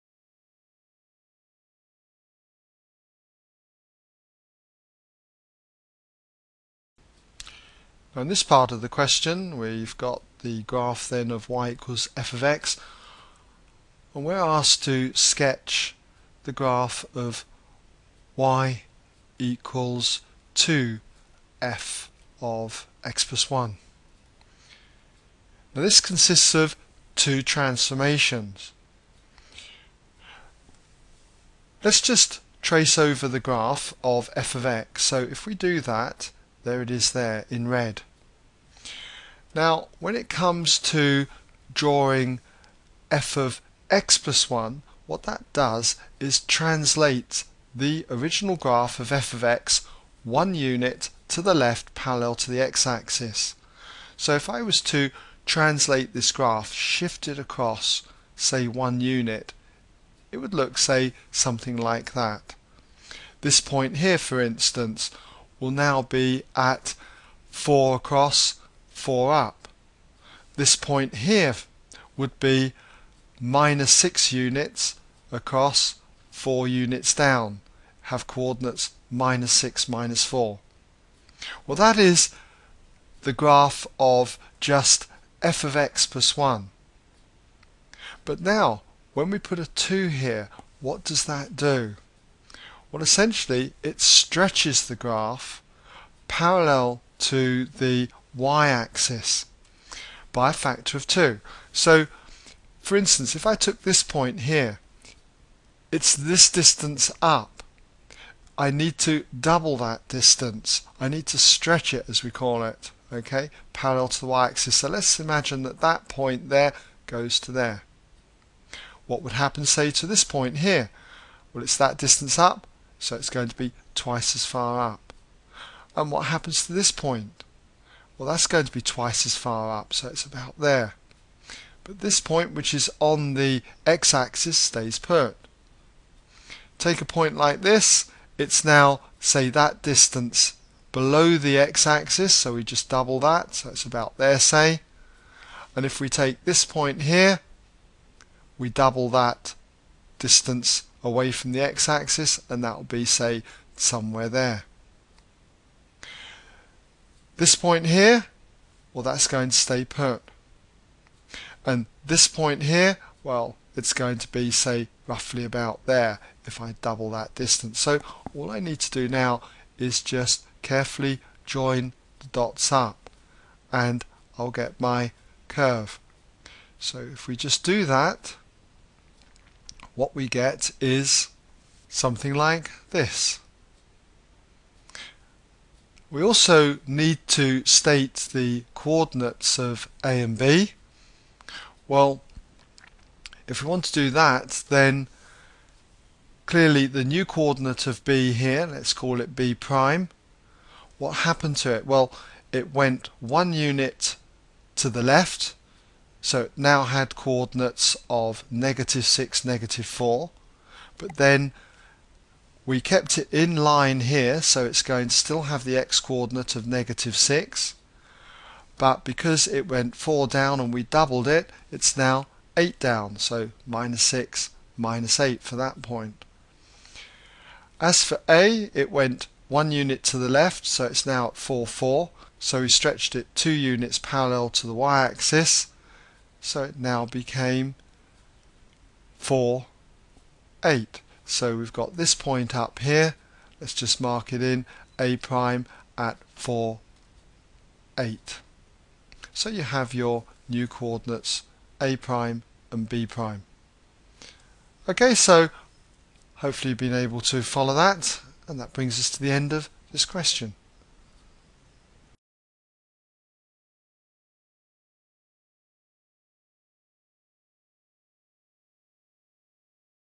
A-Level Maths Edexcel C3 January 2008 Q4c ExamSolutions
Worked solution to the above question. To see the question goto ExamSolutions